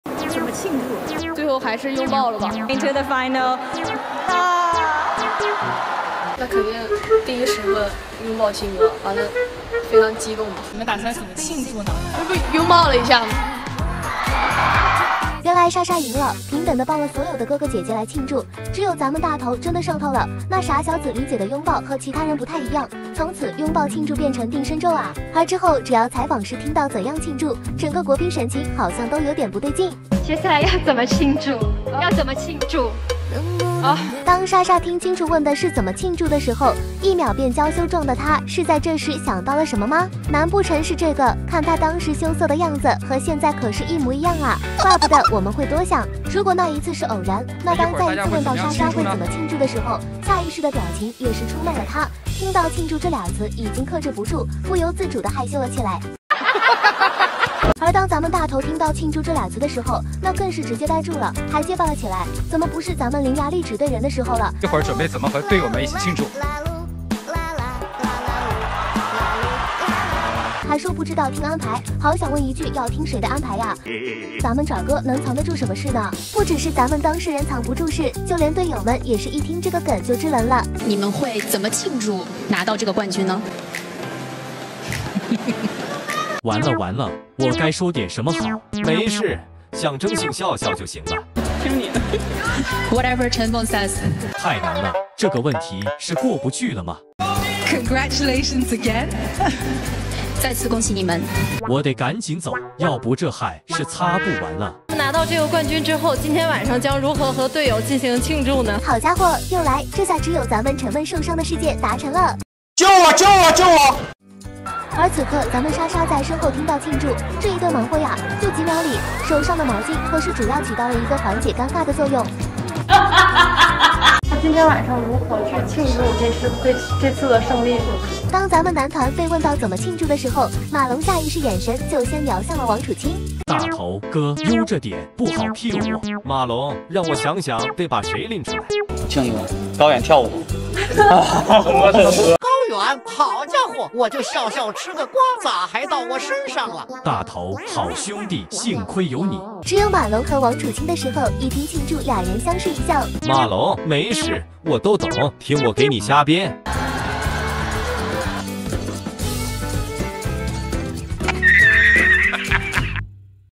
【孫穎莎x王楚欽】原來莎莎平等的抱了所有的哥哥姐姐，只有大頭真的上頭了，哈哈！！
小桃兔兔為大家帶來更多歡樂